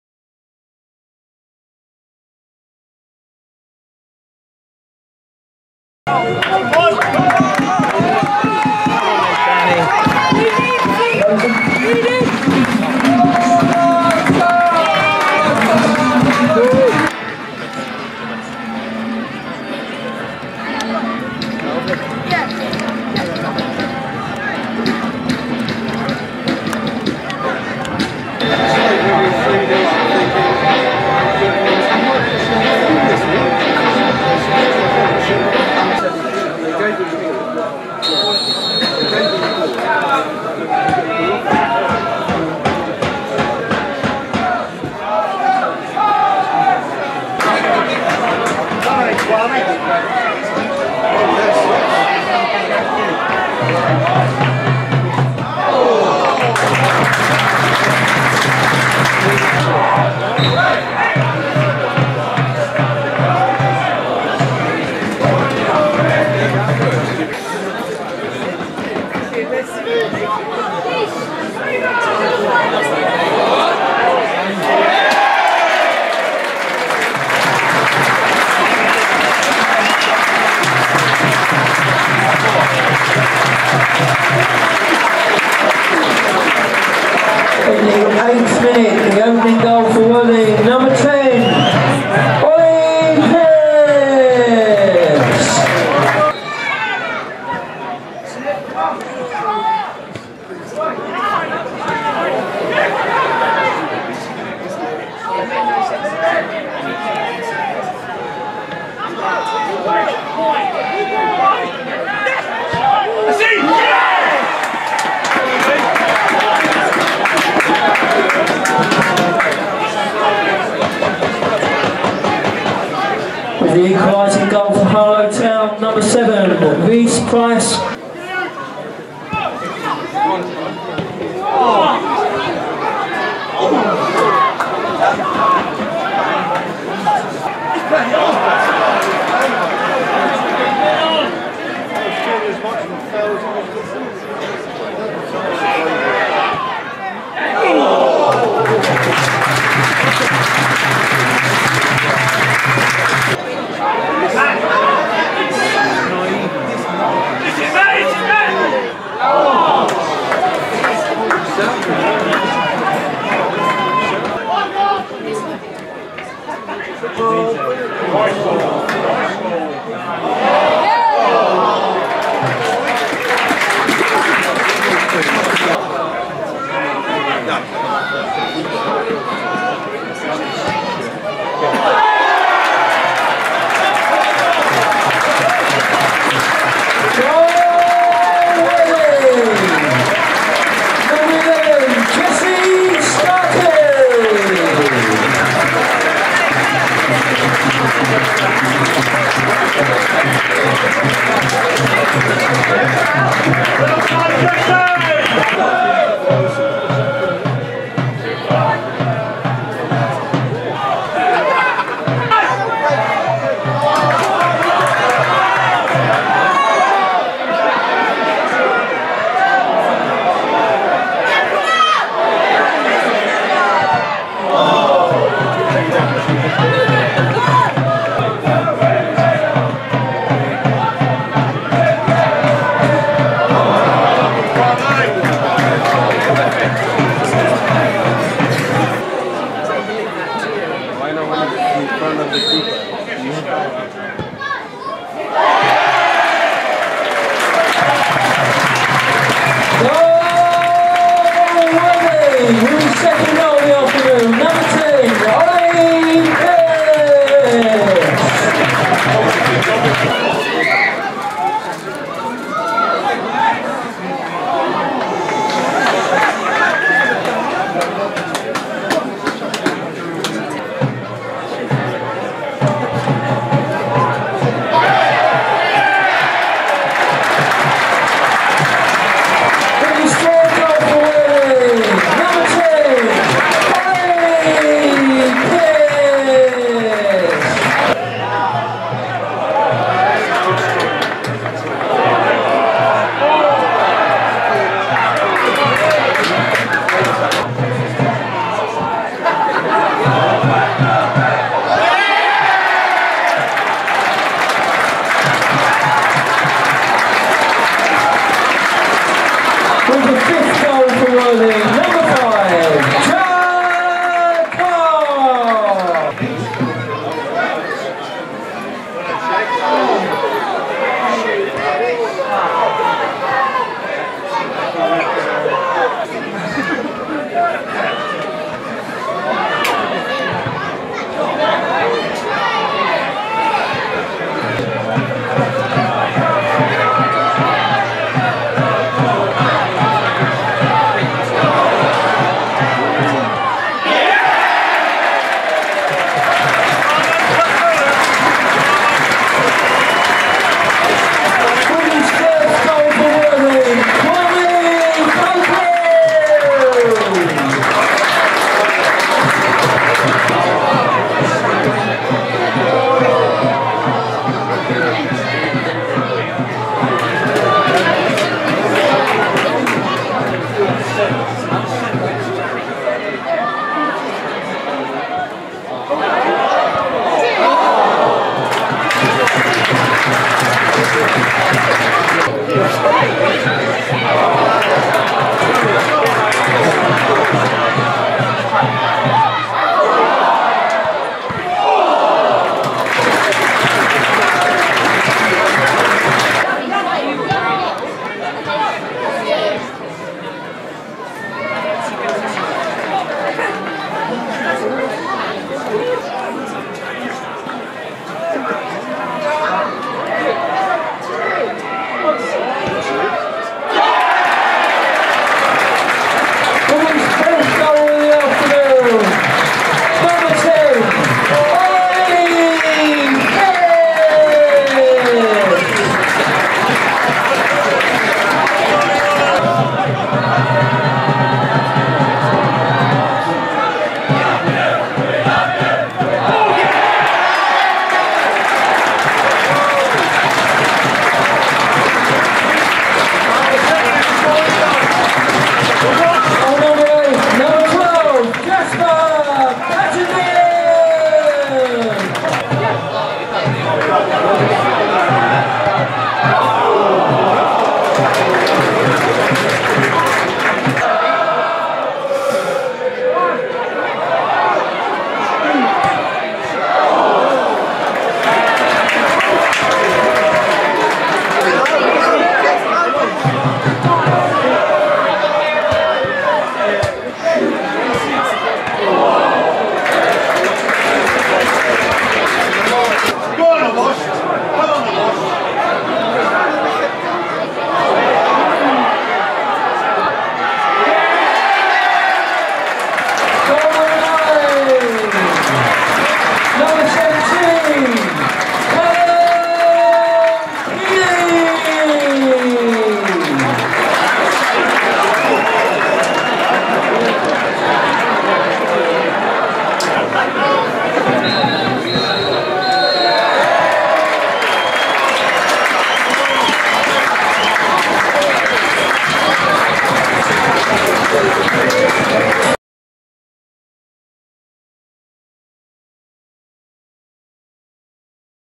Highlights: Worthing 9-1 Harlow Town - 16-2-2019
Highlights of the Bostik League Premier Division match between Worthing and Harlow Town played at the Bibby Financial Services Stadium on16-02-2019